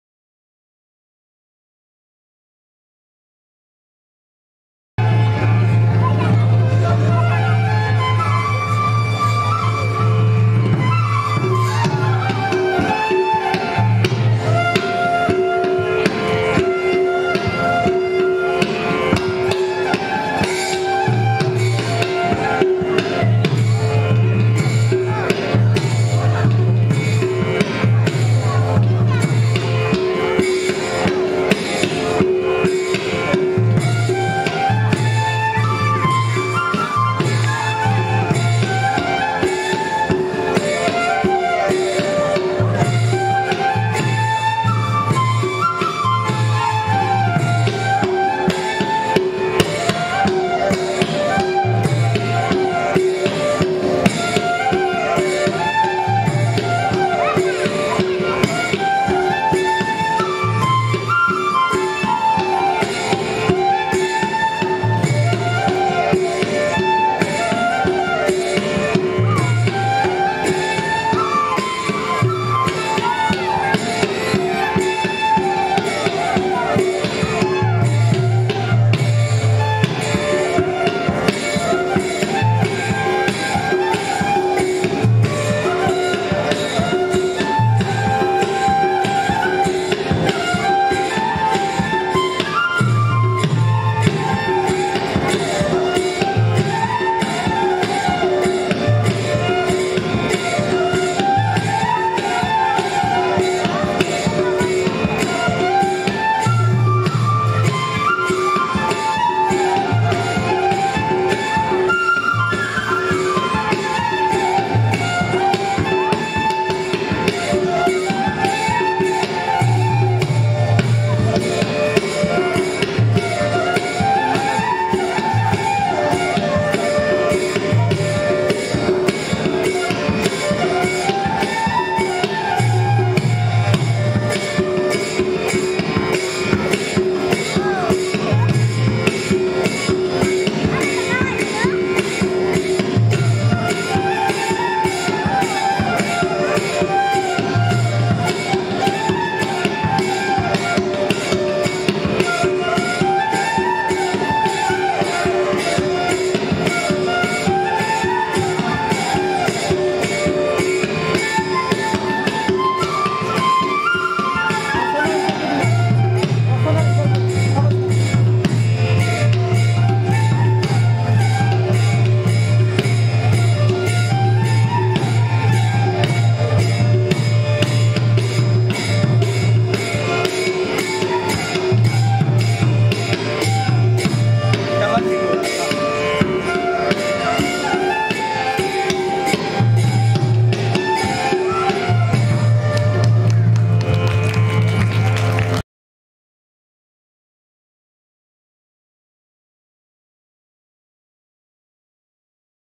Sattriya Dance
Performed at Chhaygaon Srimanta Sankardev Kala-Kristi Bikash Kendra on the occasion of Srimanta Sankardev's 570th birth anniversary.